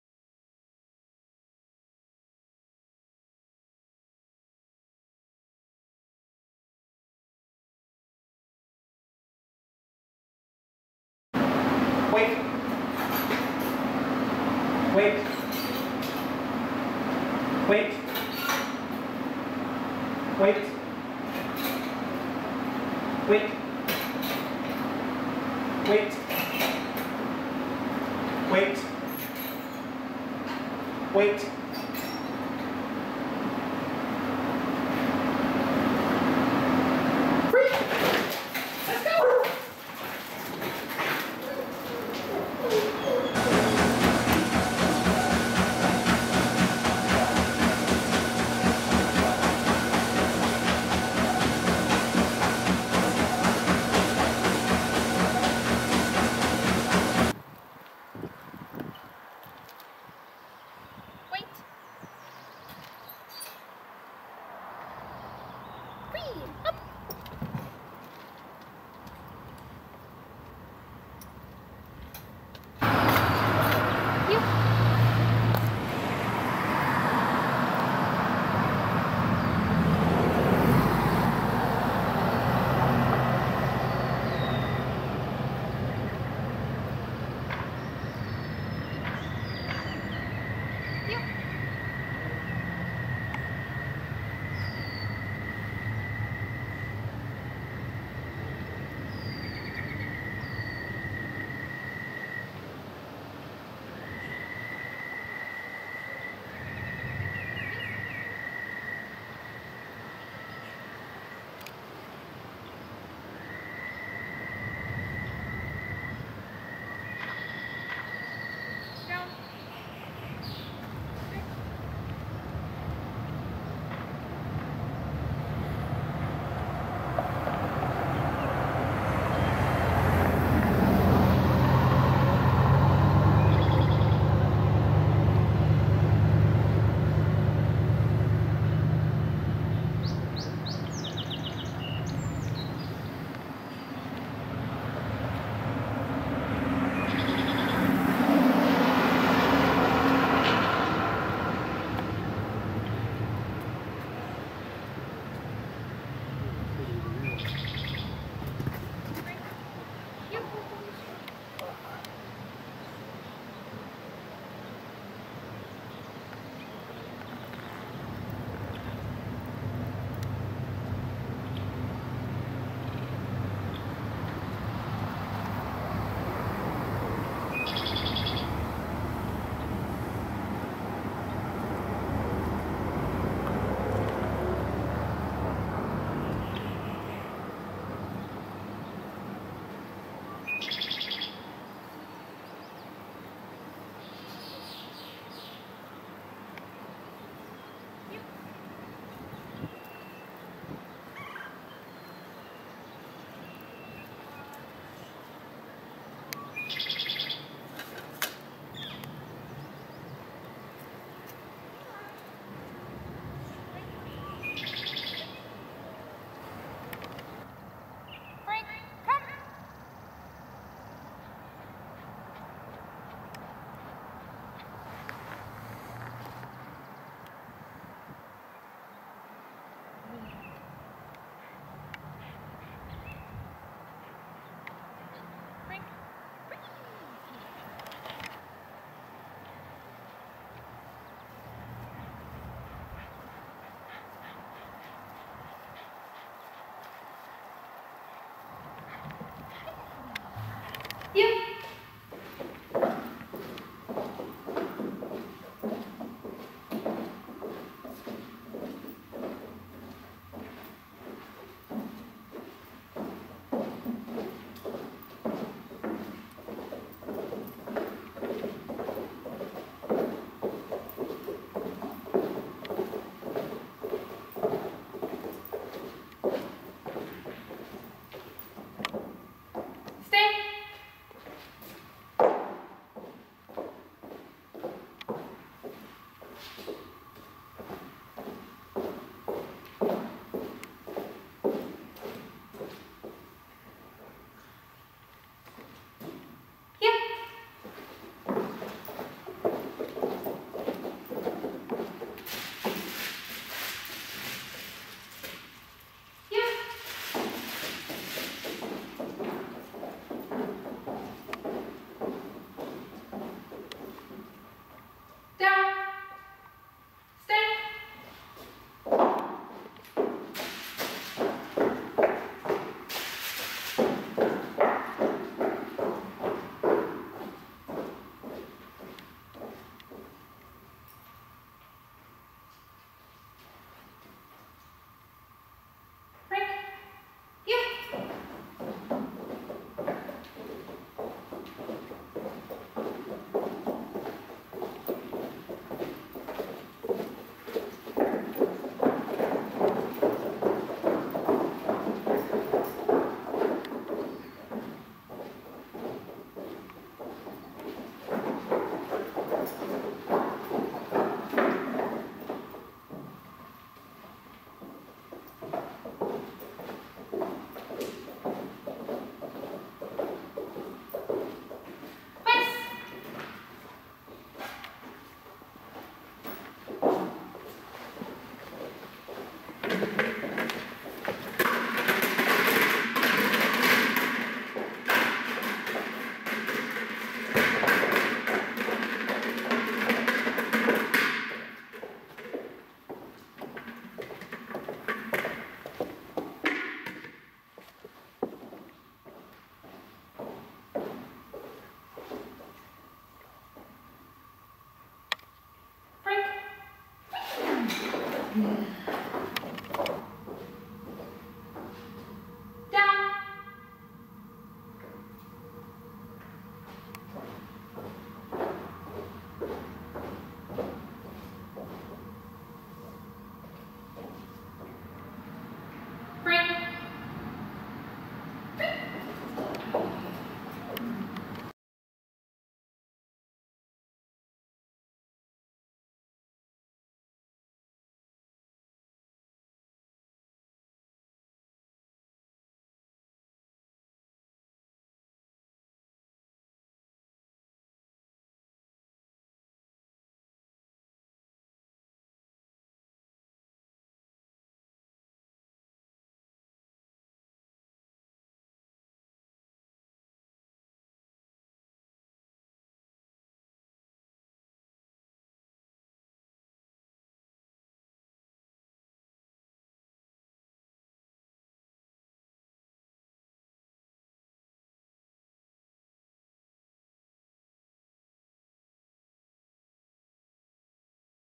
Frank - Puppy Camp Dog Training Demonstration
Frank graduated from the dog training boot camp at Neuman K-9 Academy. This program included obedience commands to sit, stay, heel or walk on a loose leash, come when called, proper etiquette, no jumping up, meeting and greeting people under control, and running on a treadmill.

Our dog training camp provides programs for all breeds such as boot camp, obedience training, and puppy camp.